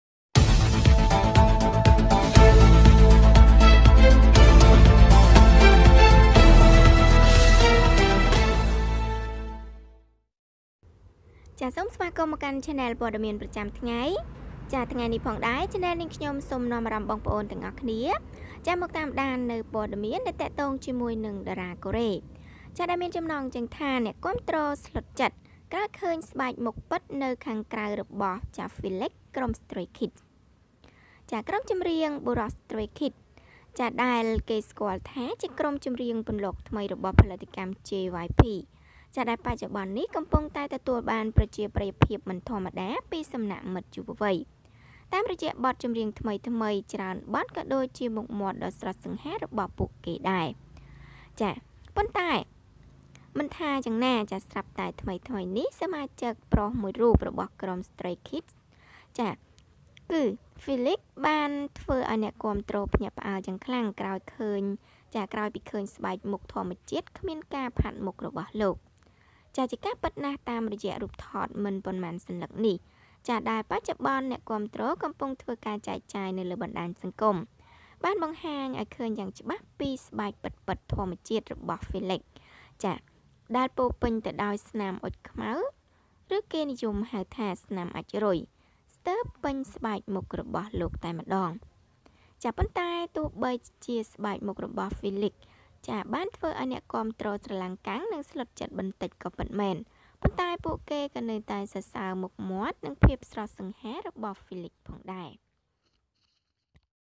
អ្នកគាំទ្រ ស្លុតចិត្ត ក្រោយឃើញស្បែកមុខពិតនៅខាងក្រៅ របស់ Felix ក្រុម Straykids!
Stupid supporters saw Felix's exotic face outside Straykids!

Thank You So much !